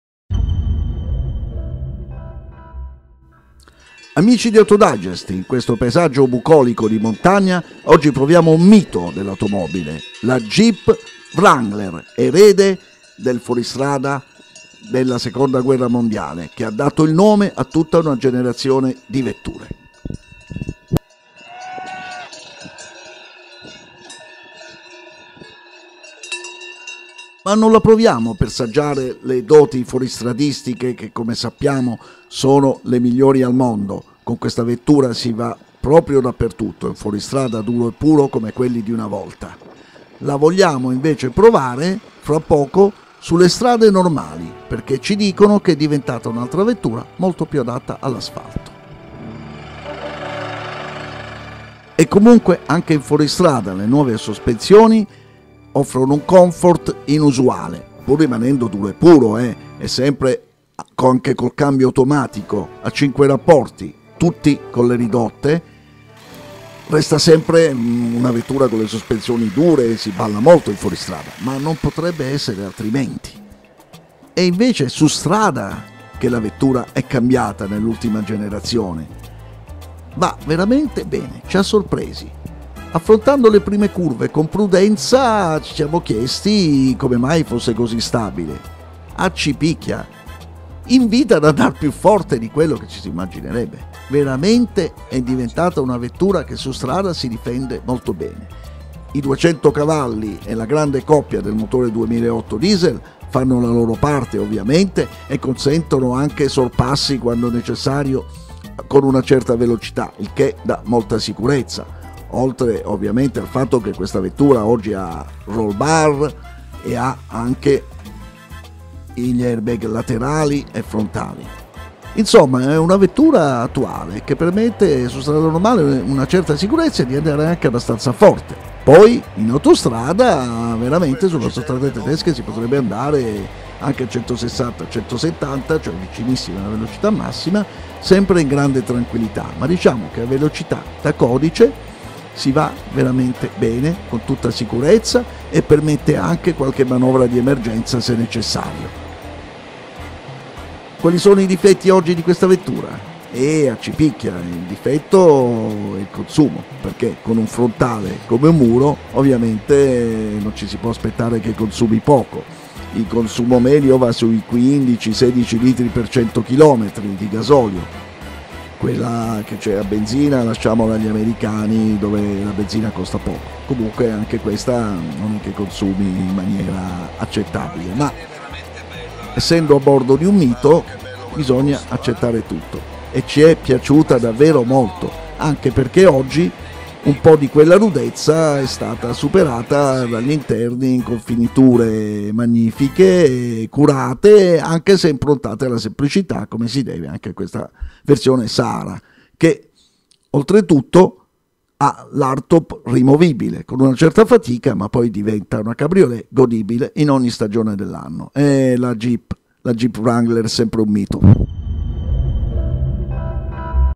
JEEP WRANGLER 2.8 CRDI : TEST DRIVE del MITO DEI FUORISTRADA
AUTO DIGEST TV HA PROVATO L'ULTIMA GENERAZIONE DELLA JEEP PER ANTONOMASIA, CON MOTORE 2.8 TURBODIESEL 4 CILINDRI 16 VALVOLE DA 200CV E 460 NM DI COPPIA, CAMBIO AUTOMATICO A 5 MARCE CON RIDUTTORE, TRAZIONE POSTERIORE CON L'INTEGRALE INSERIBILE.  LA VERSIONE SAHARA HA L?HARDTOP RIMOVIBILE; FINITURE INTERNE SOBRIE MA CURATE CON SELLERIA IN PELLE, NAVIGATORE E SISTEMA U-CONNECT, AIRBAG FRONTALI E LATERALI. PE LA SICUREZZA C'E' IL SISTEMA ANTIRIBALTAMENTO ELETTRONICO E UN ROBUSTO ROLLBAR.  MA E' SOPRATTUTTO SU STRADA E IN AUTOSTRADA CHE CI HA STUPITI PE LE DOTI DI SICUREZZA E STABILITA' DI QUESTA ULTIMA GENERAZIONE. LE DIMENSIONI SONO COMPATTE: 4,22x1,87x1,80 m. IL COMSUMO E' ADEGUATOALLE CARATTERISTICHE, OVVERO ELEVATO. IL PREZZO IN ITALIA PARTE DA 40.350 EURO, 44.200 PER L'ESEMPLARE PROVATO.